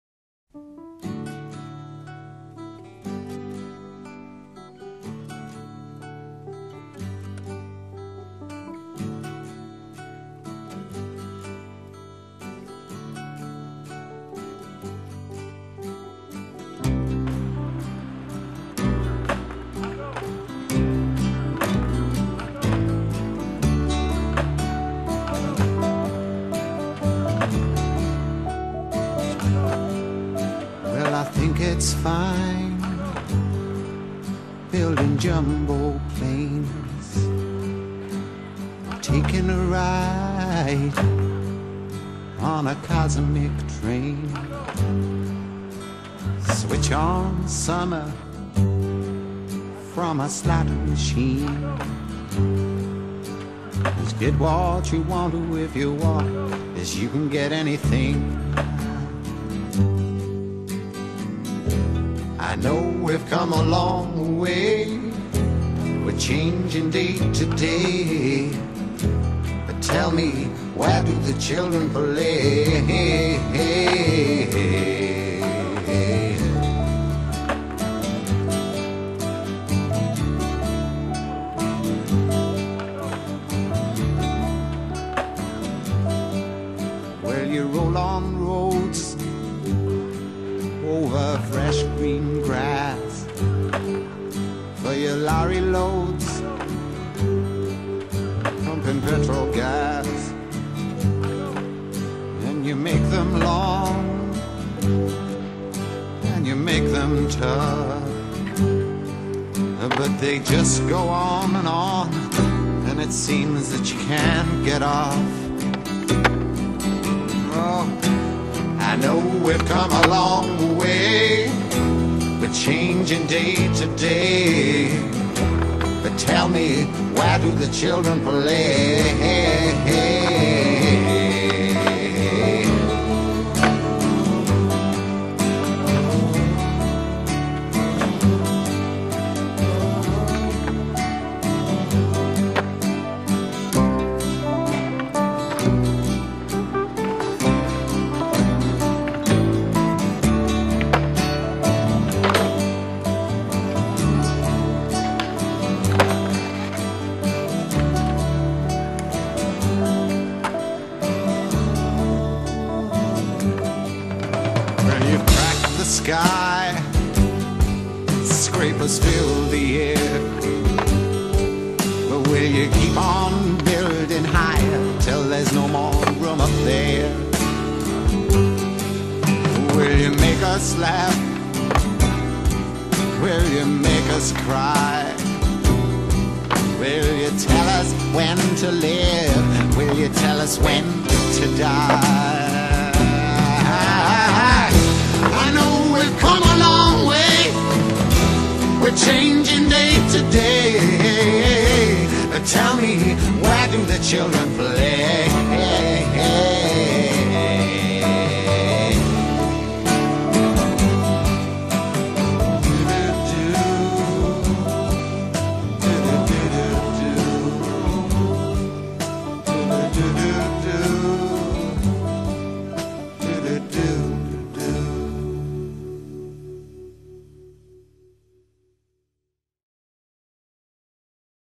Tribute to PARAL-LEL in Barcelona
This is my tribute video to PARAL-LEL, probably one of the best skate spots in Barcelona, Spain that got destroyed in November 2014... RIP